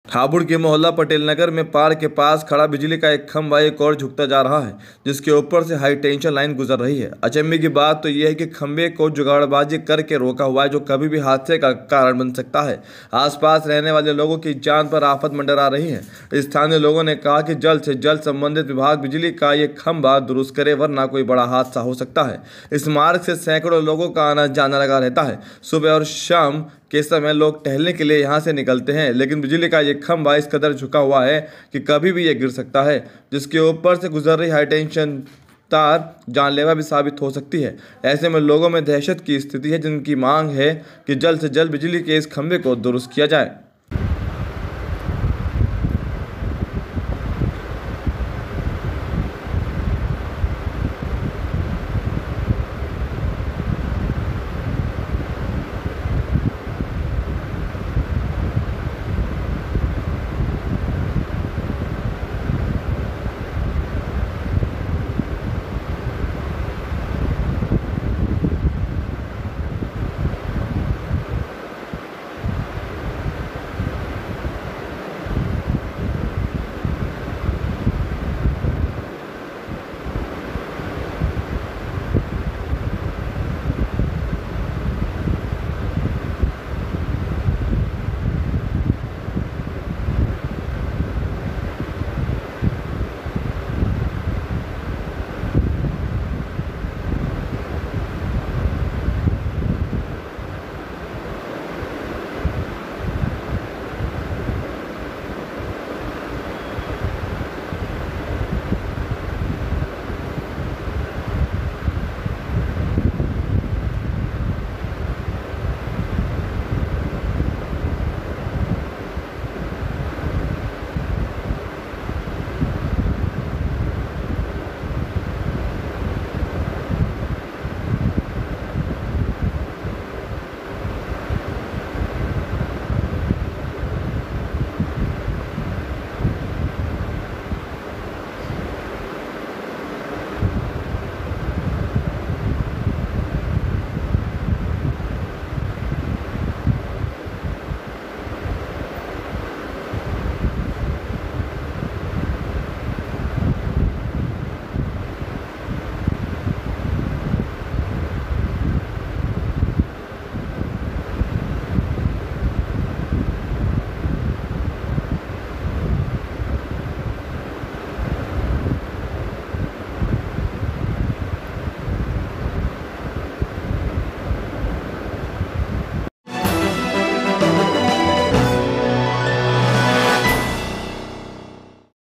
हापुड़: जुगाड़बाजी पर टिका बिजली का झुका खंभा
हापुड़ के मोहल्ला पटेल नगर में पार्क के पास खड़ा बिजली का एक खंभा एक ओर झुकता जा रहा है जिसके ऊपर से हाई टेंशन लाइन गुजर रही है। अचंबे की बात तो यह है कि खंभे को जुगाड़बाजी करके रोका हुआ है जो कभी भी हादसे का कारण बन सकता है। आसपास रहने वाले लोगों की जान पर आफत मंडरा रही है। स्थानीय लोगों ने कहा कि जल्द से जल्द संबंधित विभाग बिजली का यह खंबा दुरुस्त करें वरना कोई बड़ा हादसा हो सकता है।
इस मार्ग से सैकड़ो लोगों का आना जाना लगा रहता है। सुबह और शाम के समय लोग टहलने के लिए यहां से निकलते हैं लेकिन बिजली का यह खंबा इस कदर झुका हुआ है कि कभी भी यह गिर सकता है जिसके ऊपर से गुजर रही हाई टेंशन तार जानलेवा भी साबित हो सकती है। ऐसे में लोगों में दहशत की स्थिति है जिनकी मांग है कि जल्द से जल्द बिजली के इस खंबे को दुरुस्त किया जाए।